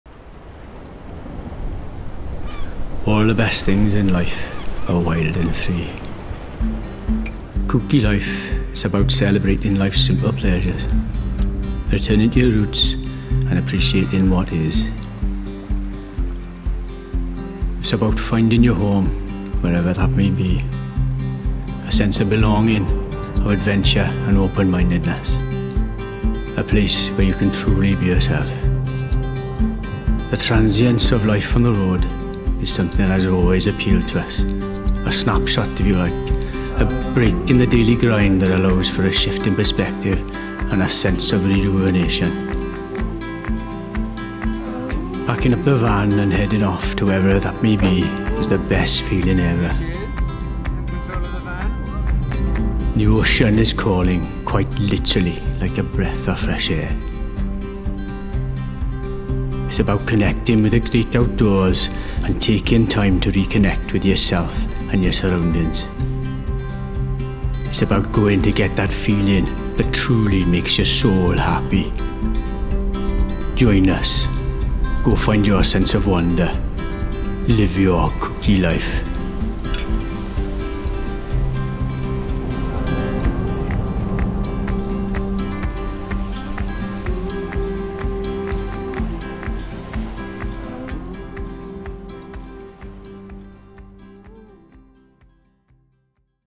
A Kooky Life // South Wales 2020
A short film highlighting what "Kookylife" is all about. Travelling across South Wales in a VW. Bliss.

Camera: Panasonic S1